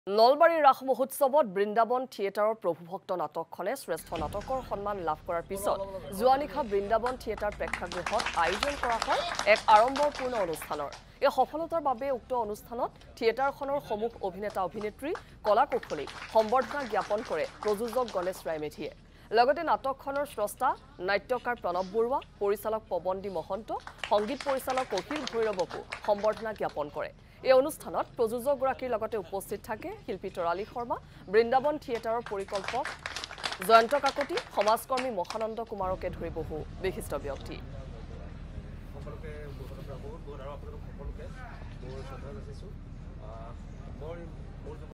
বৃন্দাবন থিয়েটাৰ প্ৰেক্ষাগৃহত আড়ম্বৰপূ্ৰ্ণ অনুষ্ঠান...
বৃন্দাবন থিয়েটাৰ প্ৰেক্ষাগৃহত আড়ম্বৰপূ্ৰ্ণ অনুষ্ঠান। সমূহ অভিনেতা-অভিনেত্ৰী, কলা-কুশলীক সম্বৰ্ধনা। প্ৰযোজক গণেশ ৰায় মেধিয়ে প্ৰদান কৰে অভিনন্দন পত্ৰ।